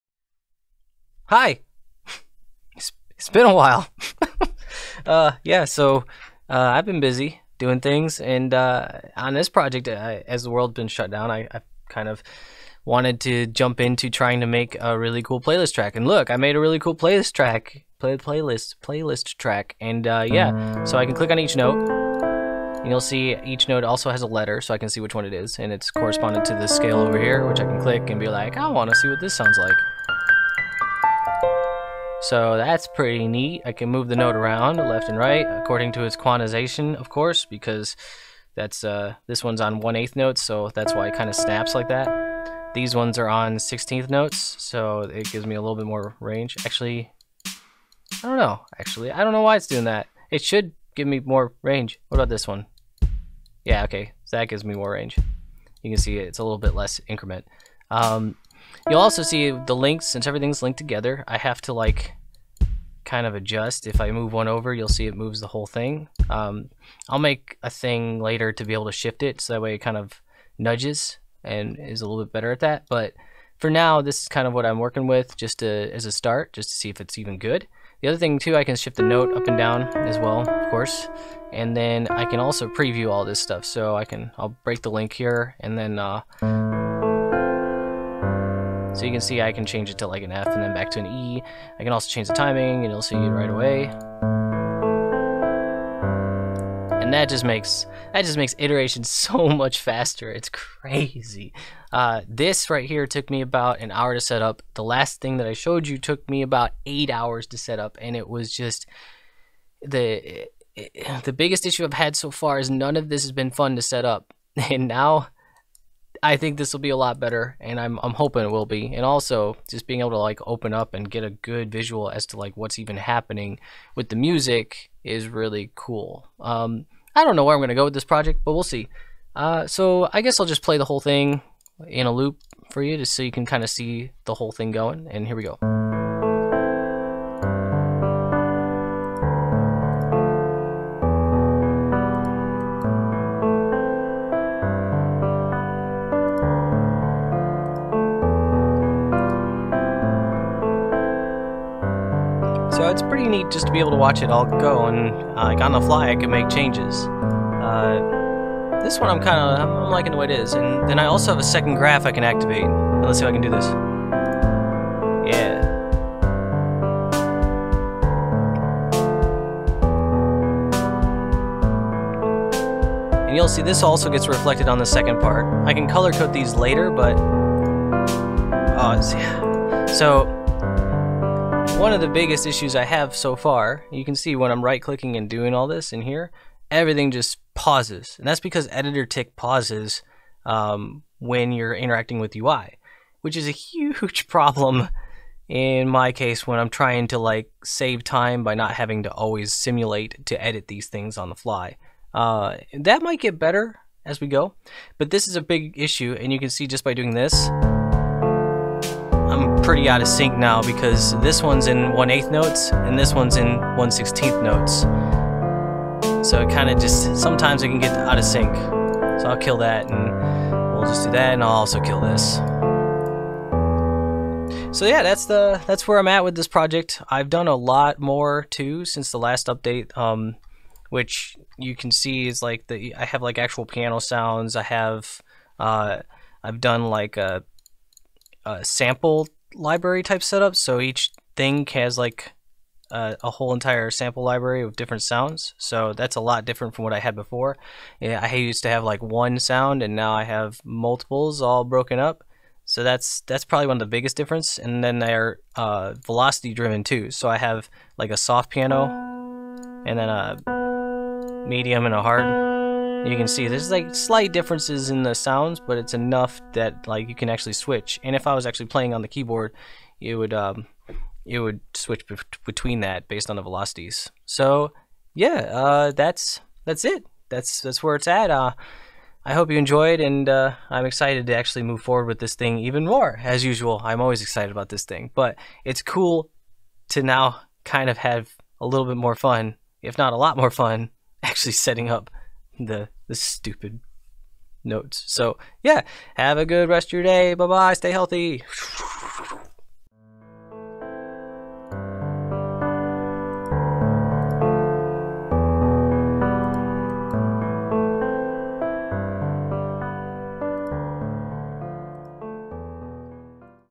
Editor Utility Widget Midi-Style Playlist! Project Mix
Here is the first form of a midi-style playlist editor for creating and editing compositions in unreal. It's kinda nuts and finally might make this more fun to actually create content for. We shall see. Anyhow, enjoy the shenanigans and have a great day!